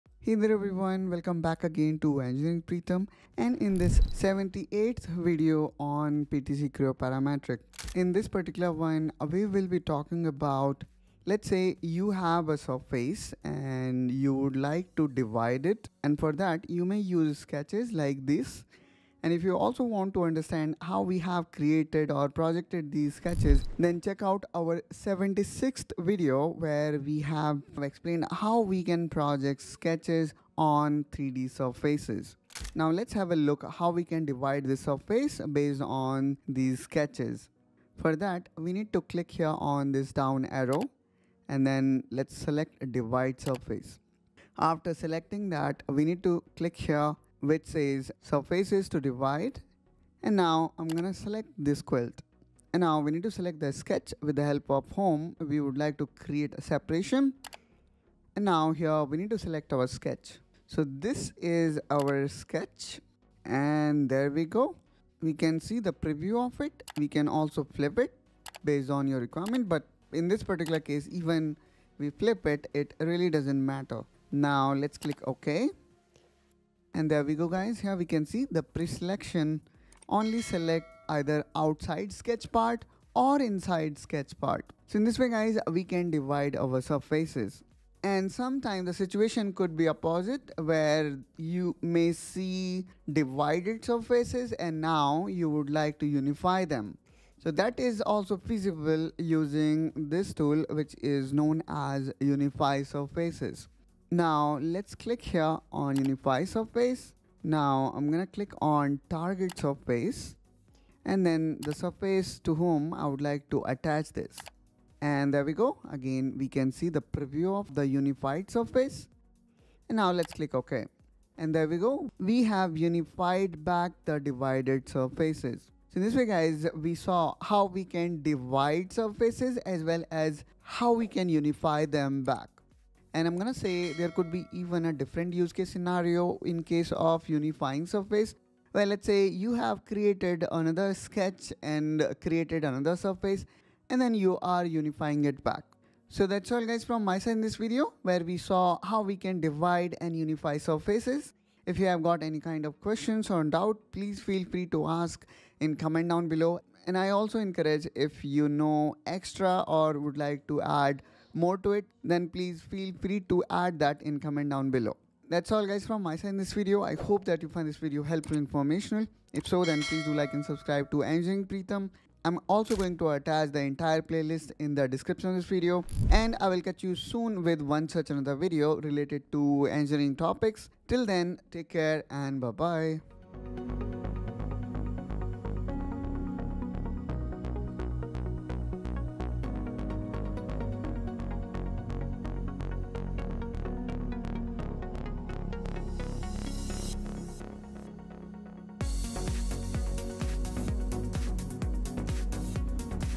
Divide and Unify Surfaces in Creo Parametric | Surface Modeling Tutorial | #78
In this video, we will show you how to divide and merge surfaces in PTC Creo using sketches. You’ll learn step by step how to use the Divide Surface and Unify Surface tools to separate or join surfaces easily.

helps us create more content like this!